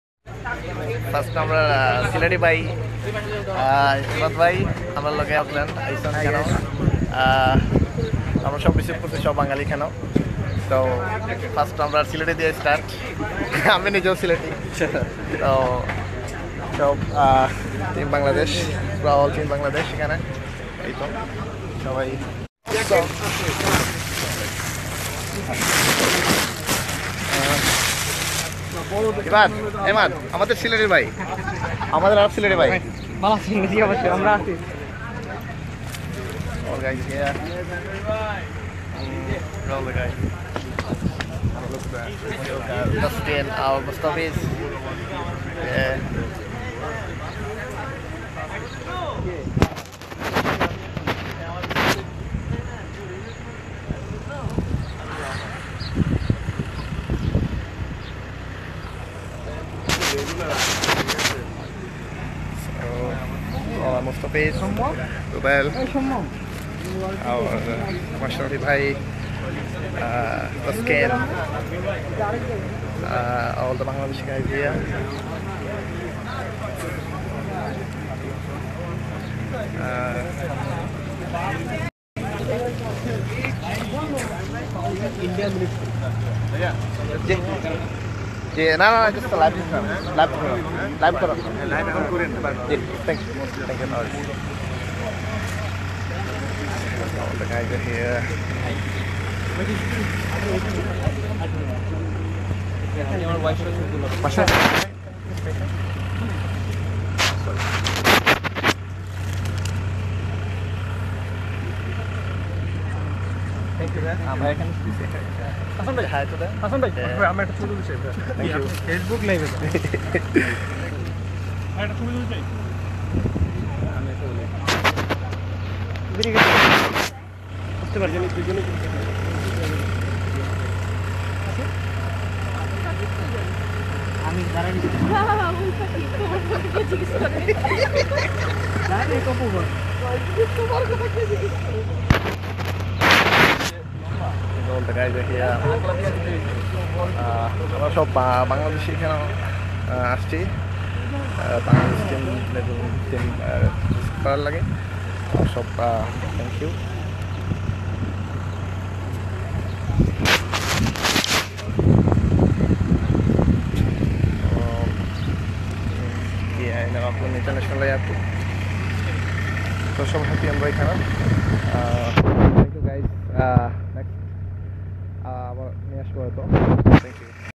Bangladesh Cricket team Host by Nahidul Islam and community In Auckland ,New zealand
Bangladesh team Host by Nahidul Islam in Auckland Airport ,New zealand 2016